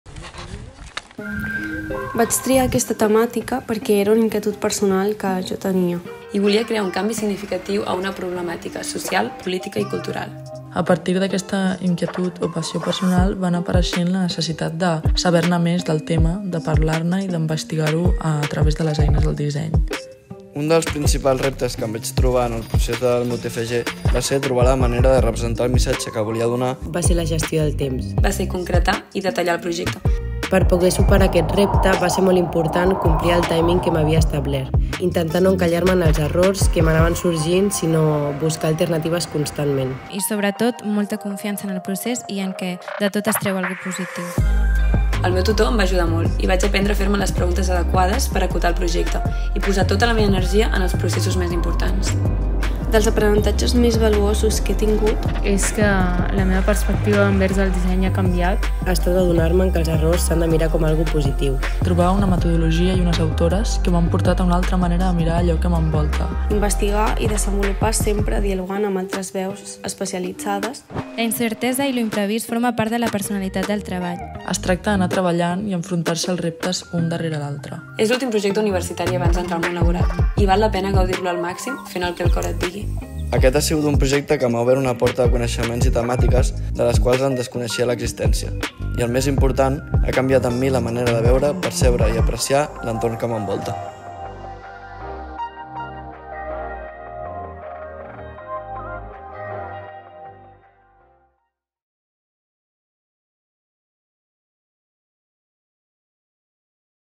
BAU Talent Visions. Grau en Disseny: estudiants
BAU Talent Visions és una mostra de Treballs Finals del Grau en Disseny.
L'alumnat de la promoció 2022-23 del Grau en Disseny parla sobre l'experiència de desenvolupar els seus TFG: inquietuds, reptes, aprenentatges...

BAU Talent Visions es una muestra de Trabajos Finales del Grado en Diseño.
El alumnado de la promoción 2022-23 del Grado en Diseño de Moda habla sobre la experiencia de desarrollar sus TFG: inquietudes, retos, aprendizajes...